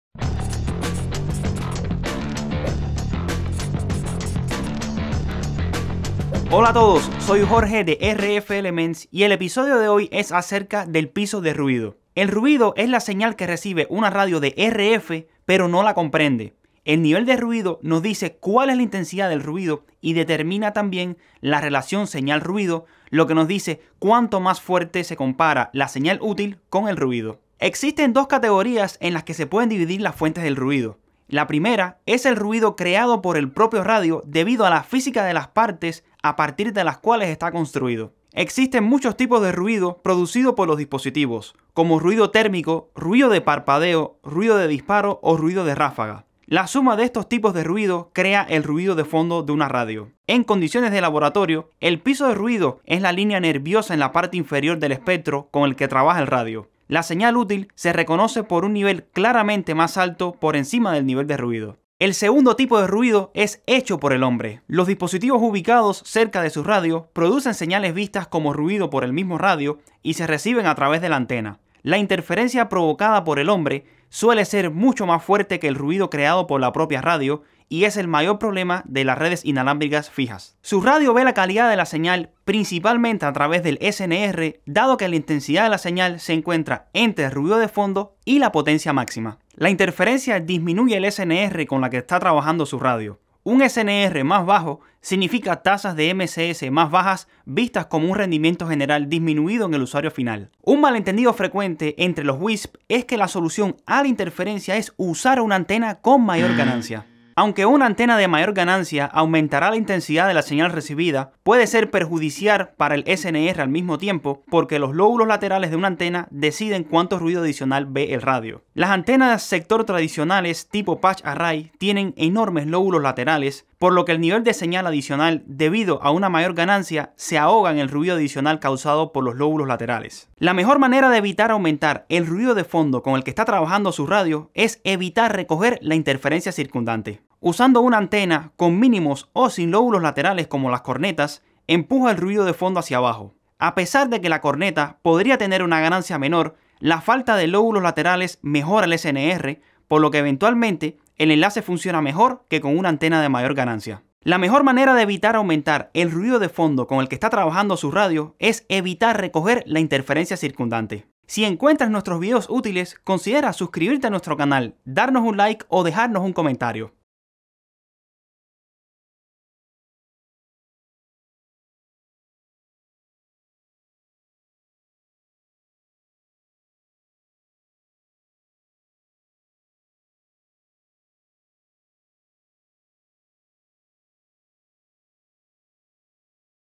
Inside Wireless en Español: Piso de Ruido
En este episodio de Inside Wireless hablamos del piso de ruido (PR). El ruido es la señal que recibe una radio de RF, pero no la entiende. El piso de ruido nos dice cuál es la fuerza del ruido en un ancho de banda dado y determina la relación señal / ruido (SNR). SNR nos dice cuánto más fuerte es la señal útil en comparación con el ruido.

¿Quiere mantener bajo el ruido de su red inalámbrica? Compruebe nuestras galardonadas antenas que le permiten RECHAZAR EL RUIDO, AHORRAR ESPECTRO y CRECER INTELIGENTE:

Hay dos categorías en las que se pueden dividir las fuentes de ruido:

1. Ruido creado por la propia radio debido a la física de las piezas con las que está construida. Hay muchos tipos de ruido que producen los dispositivos, como ruido térmico, ruido de parpadeo, ruido de disparo o ruido de ráfaga. La suma de todos estos tipos de ruido crea el ruido de fondo de una radio. En condiciones de laboratorio, el piso de ruido es la línea de nerviosismo en la parte inferior del espectro con el que trabaja la radio. La señal útil se reconoce por un nivel claramente más alto por encima del piso de ruido.

2. Ruido provocado por el hombre. Los dispositivos ubicados cerca de su radio producen señales que su radio ve como ruido y se reciben a través de la antena. La interferencia provocada por el hombre suele ser mucho más fuerte que el ruido creado por la propia radio y es el mayor problema de las redes inalámbricas fijas.

La radio de RF ve la calidad de la señal principalmente a través de la SNR, dado que la fuerza de la señal se encuentra entre el piso de ruido y la potencia máxima. La interferencia disminuye la SNR con la que trabaja su radio. Una SNR más baja significa tasas de MCS más bajas que se ven como una disminución del rendimiento general en el extremo del usuario.

Un malentendido frecuente entre los WISP es que la solución a la interferencia es usar una antena con mayor ganancia. Aunque la antena de mayor ganancia aumentará la intensidad de la señal recibida, puede ser perjudicial para la SNR al mismo tiempo. Los lóbulos laterales de una antena deciden cuánto ruido adicional ve la radio.

Las antenas de matriz de parche de sector tradicionales tienen lóbulos laterales enormes, por lo que el nivel de señal adicional debido a una mayor ganancia se ahoga en el ruido adicional causado por los lóbulos laterales.

La mejor manera de evitar que aumente el nivel de ruido con el que trabaja su radio es evitar la acumulación de interferencias circundantes. Usando una antena sin lóbulos laterales, como una horn, empuja el piso de ruido hacia abajo, mejorando su SNR. A pesar de que la horn puede tener una ganancia menor, la falta de lóbulos laterales mejora la SNR, por lo que eventualmente el enlace funciona mejor que con una antena de mayor ganancia.

Si la interferencia en el área es fuerte, la mejora cuando se usan horns puede ser marginal, por lo que es bueno tener en cuenta que cada enlace debe tratarse como un caso separado.

0:00 - Introducción
0:12 - Qué es el ruido y SNR
0:27 - Tipos de ruido
1:36 - Interferencia
1:48 - Solución al ruido
2:42 - Conclusión